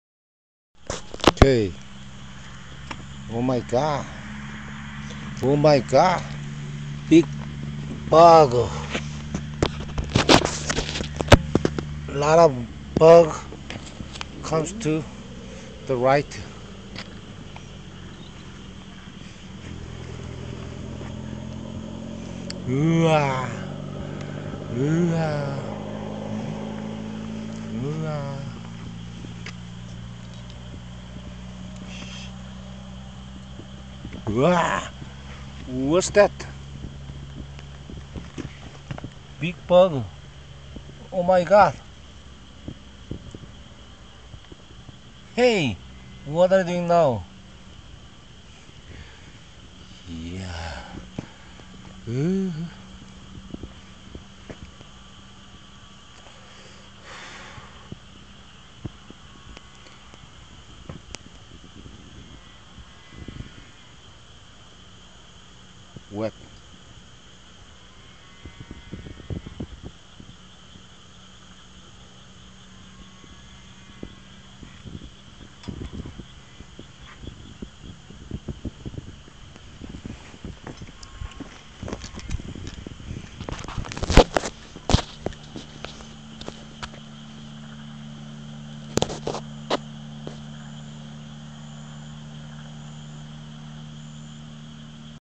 Traces on the night riding...
Whenever riding at night, many bugs hit Harley. They would gather the lights on Harley together. These traces of bugs must be removed after riding. If not do so, the dead bugs enable Harley parts rotted.